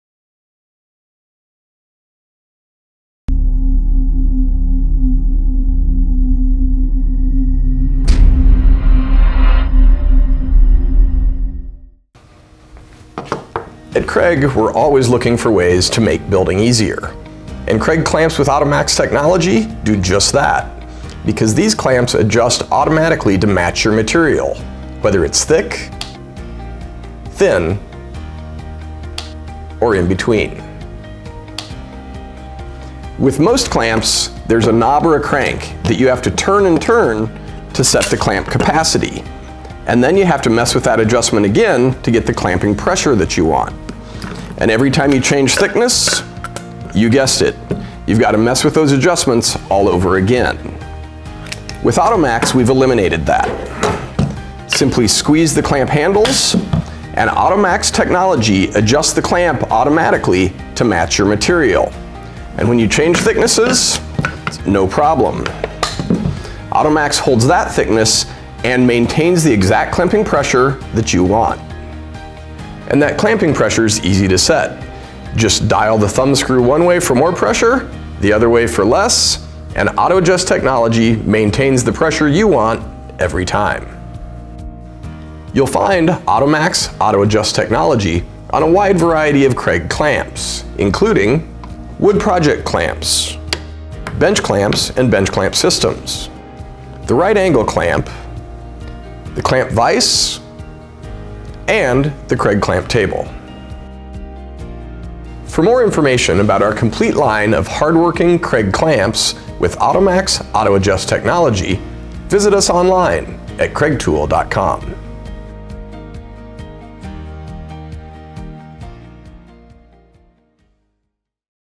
Get A Firm Grip With These Auto-Adjusting Woodworking Clamps
Kreg Clamps with Automaxx® adjust automatically to clamp materials that are thick, thin, or in between. No fussing with knobs to adjust the clamp capacity, no fiddling to get the exact pressure you want, no need for thickness adjustment at all — just powerful, predictable clamping.

Simply squeeze the clamp handles, and Automaxx® Technology adjusts the clamp automatically to the correct thickness, all while keeping the clamping pressure you desire.

You’ll find Automaxx® Auto-Adjust Technology on a wide variety of Kreg clamps, including Wood Project Clamps, Bench Clamps and Bench Clamp Systems, Auto-Adjust Bar Clamps, the Right Angle Clamp, the Clamp Vise, and the Kreg Clamp Table.